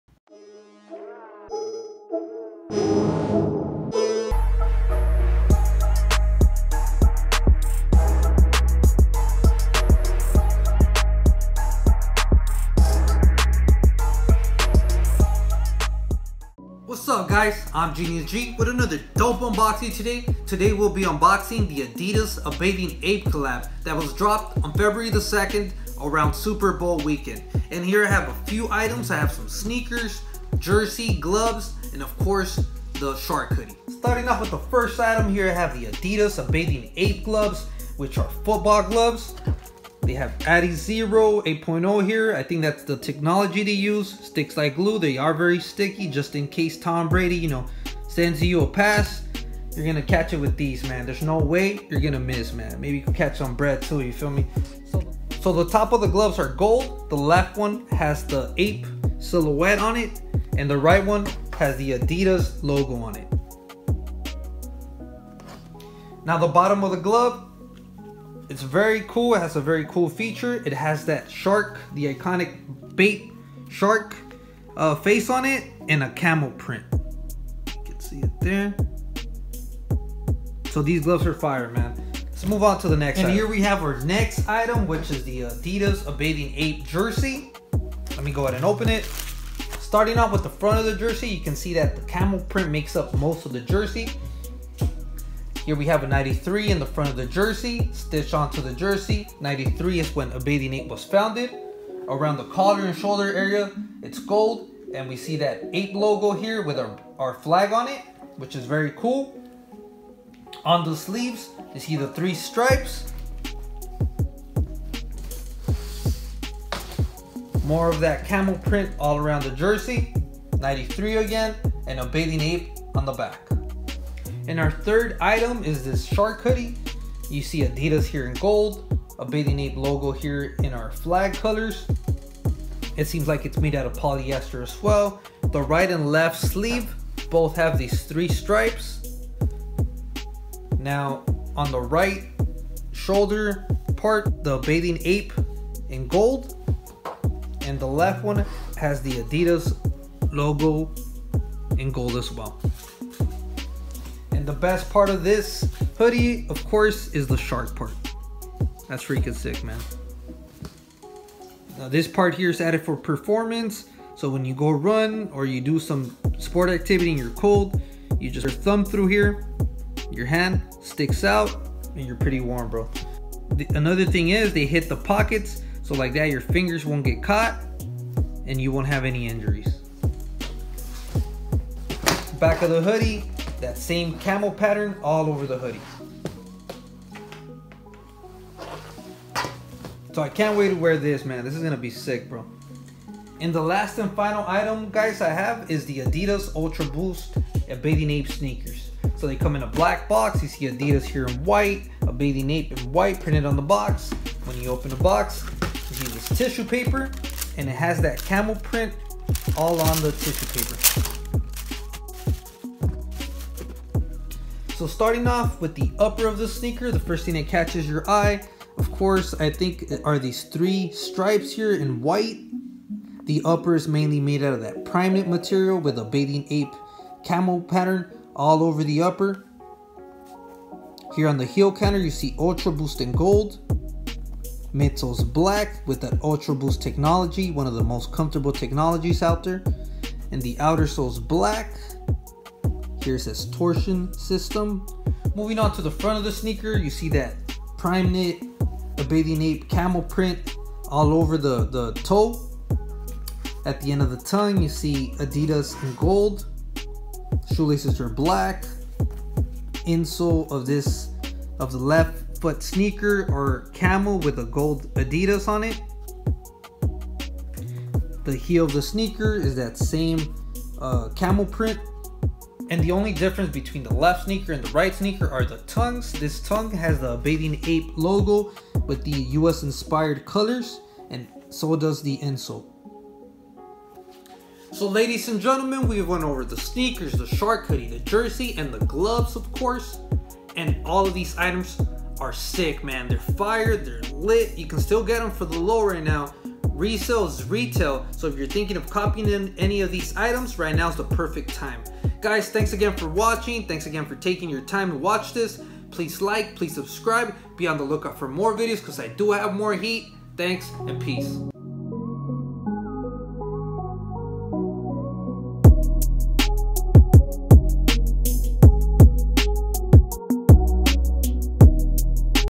ADIDAS X A BATHING APE x SHARK HOODIE x BAPE ULTRABOOST X UNBOXING
During superbowl weekend ADIDAS X A BATHING APE dropped major heat such as gloves, jerseys, cleats, helmets, etc.. let's unbox some of these items and see why they all sold out!